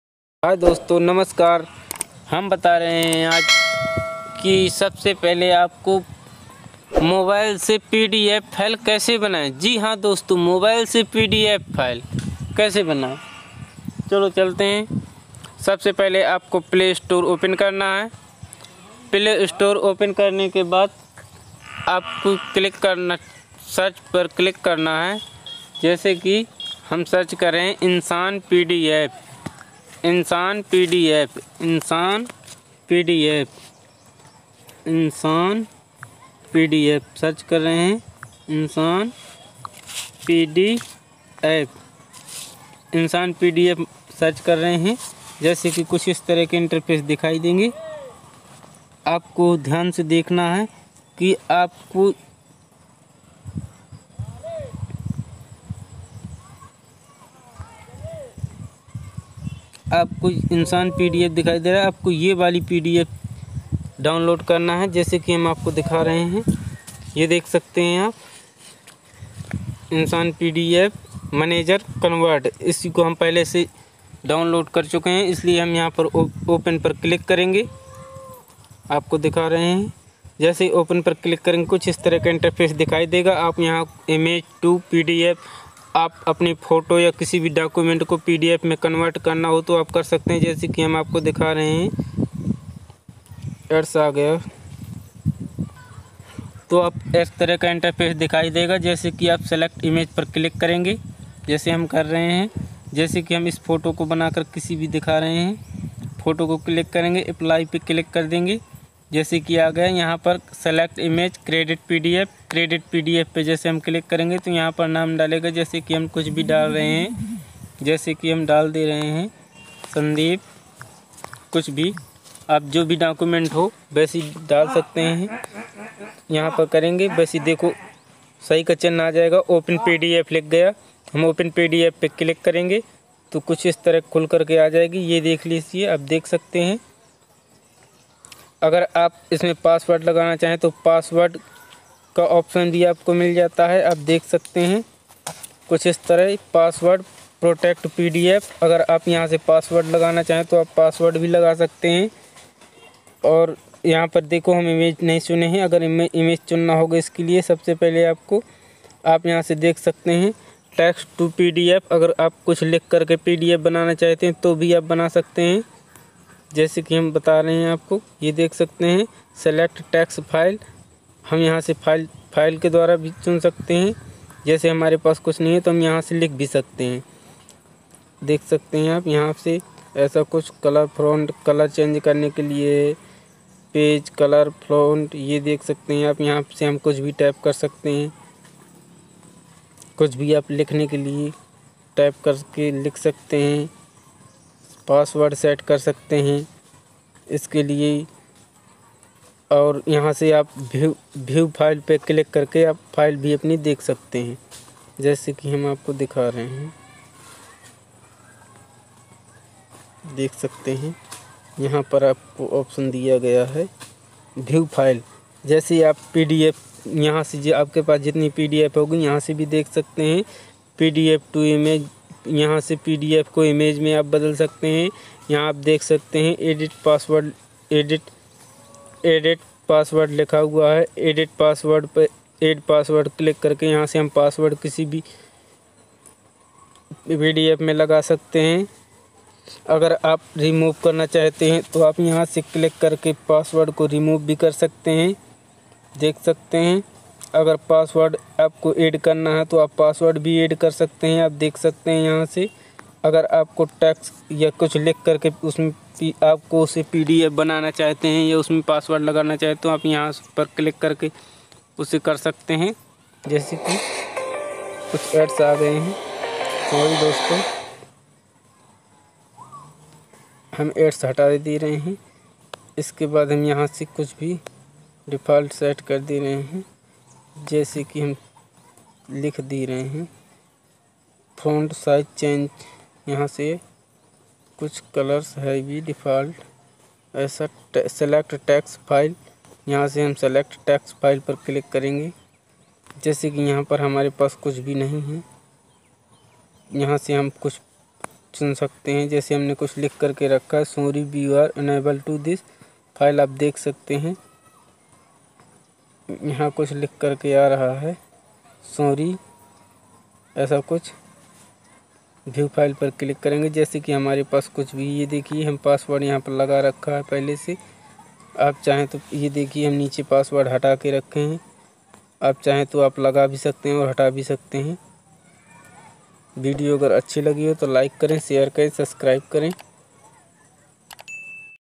PDF File kaise babaye apne mobile se पी डी एफ फाइल कैसे बनाएं अपने मोबाइल से 2022
पीडीएफ फाइल कैसे बनाएं अपने मोबाइल से 2022
रामबाबू पटेल
Rambabu patel